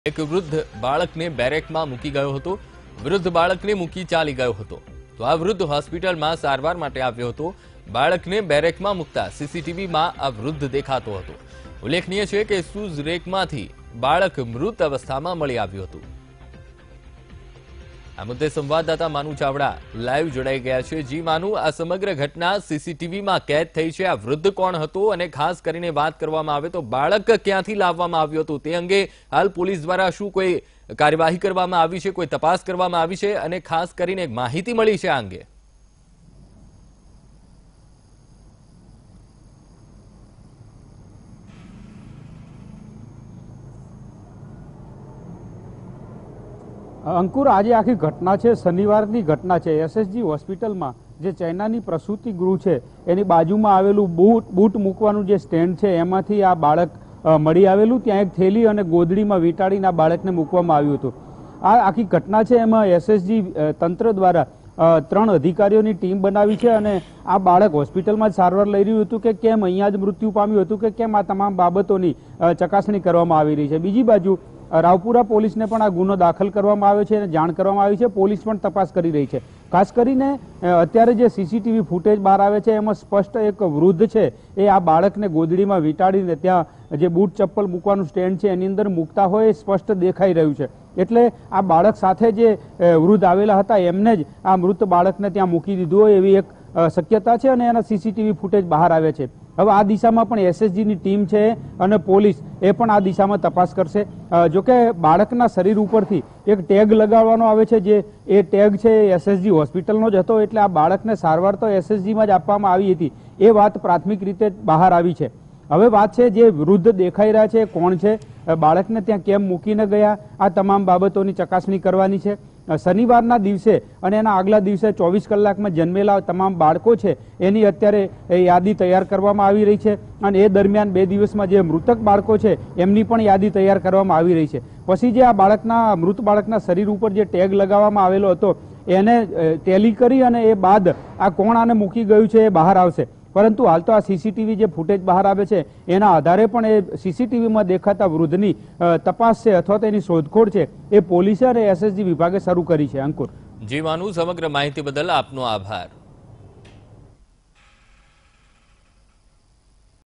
CCTV Footage of Senior Citizen leaving Dead Child in Shoe Rack of SSG Hospital, Vadodara | Vtv News
CCTV Footage of Senior Citizen leaving Dead Child in Shoe Rack of SSG Hospital, Vadodara